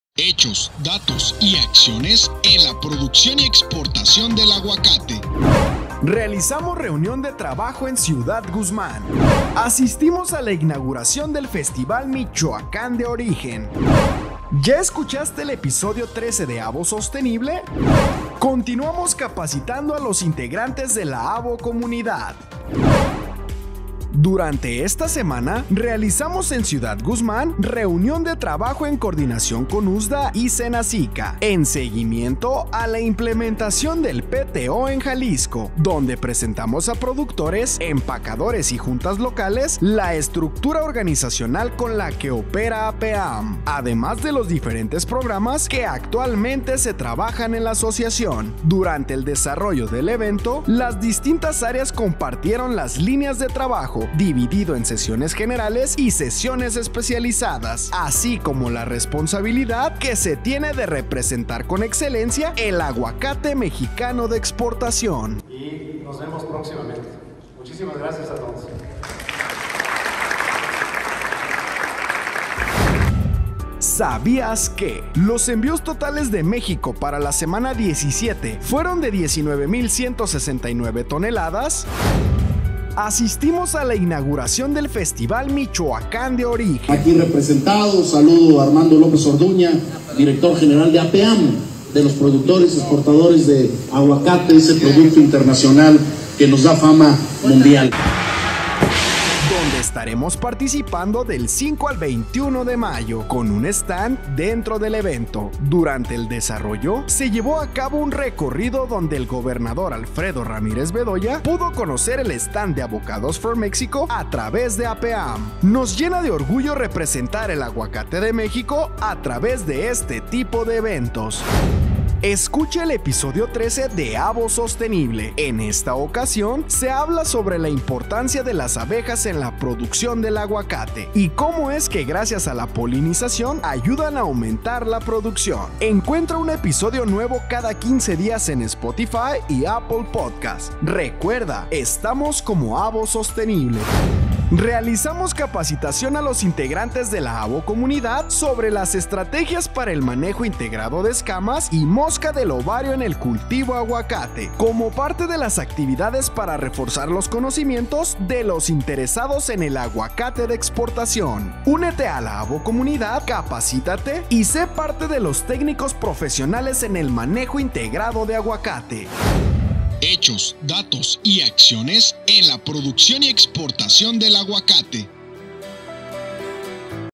Hechos, datos y acciones - Mayo semana 1
Conoce las noticias más importantes de la semana en nuestra Cápsula Informativa - Mayo Semana 1.

• Realizamos reunión de trabajo en Ciudad Guzmán.
• Asistimos a la inauguración del Festival Michoacán de Origen 2022.
• Escucha el episodio 13 de nuestro podcast Avo Sostenible.
• Capacitaciones a los integrantes de la Avocomunidad.